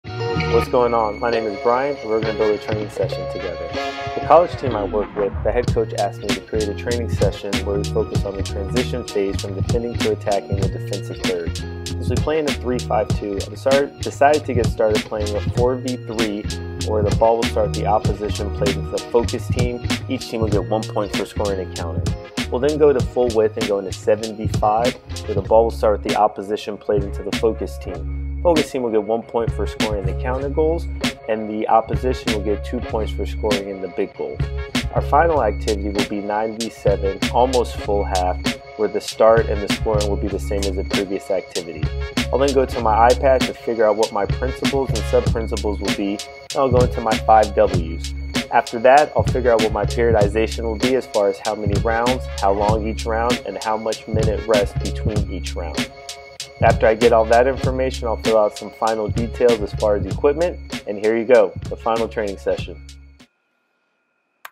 Building a Training Session Part 1 (College Level Team)
POV of me creating a training session for my college team.